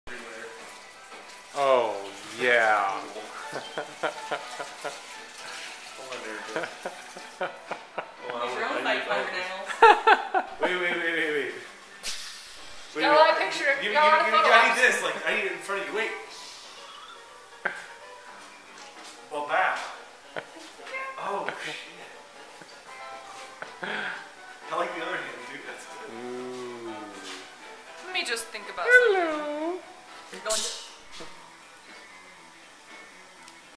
The next super hero - Crab Girl!
Transforms Alaska King Crab legs into deadly weapons!